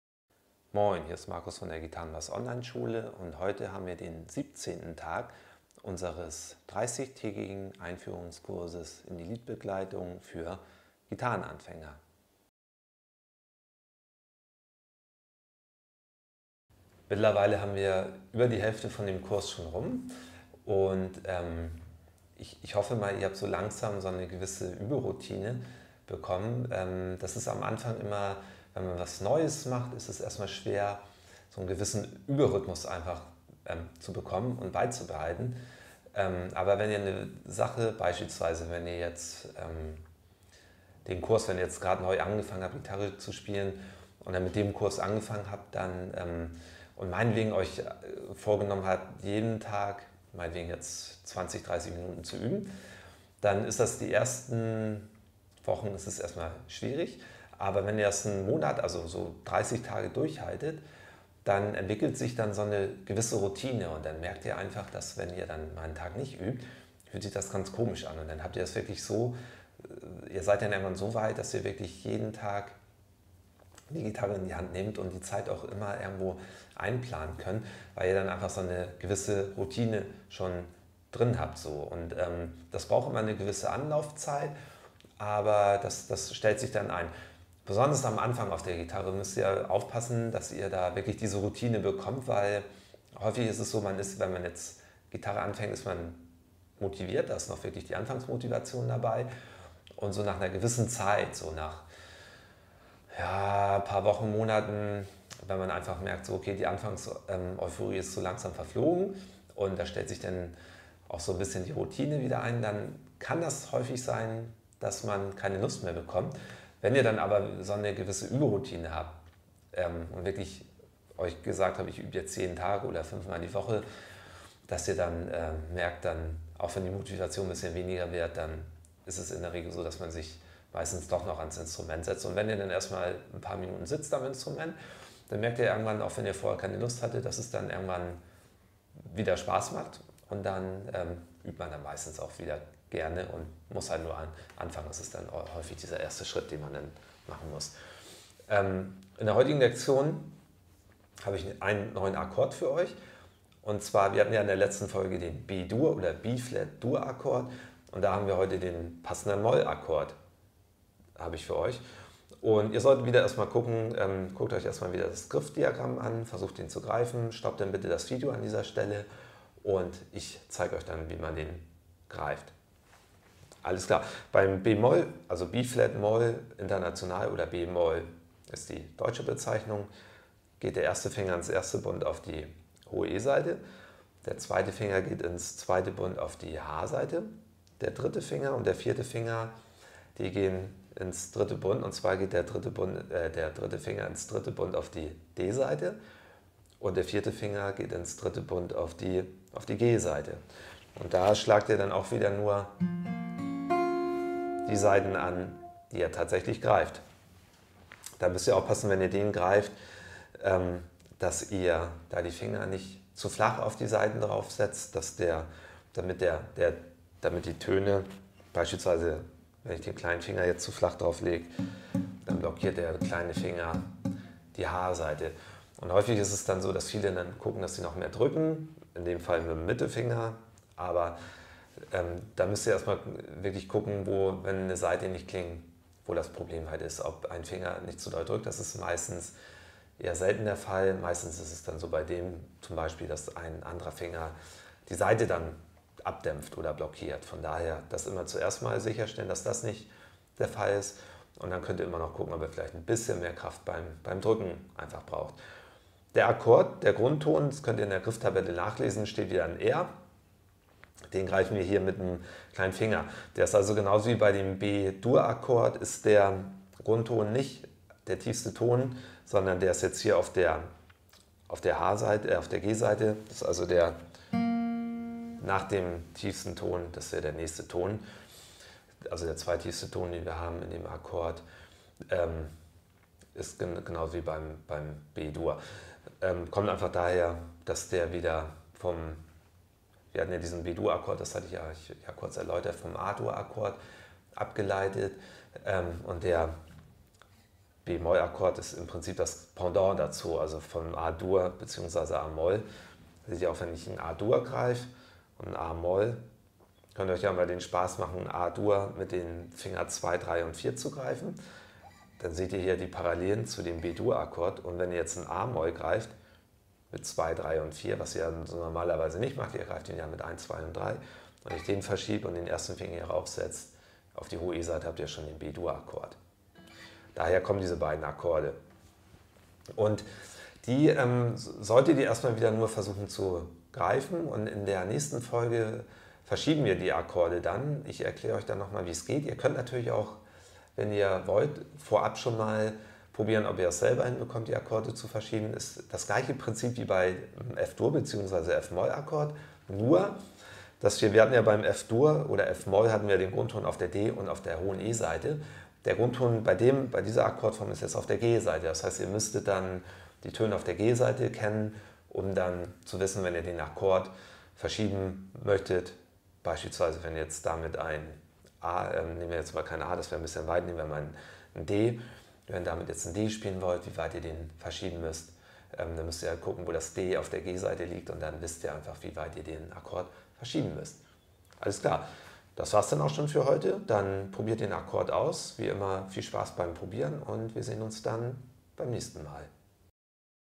Gitarre lernen für Anfänger - 30tägiger Liedbegleitungskurs - Tag 17
In der heutigen Folge des 30tägigen Einführungskurses in die Liedbegleitung für Gitarrenanfänger zeige ich euch den B-Moll- oder auch Bb-Moll-Akkord.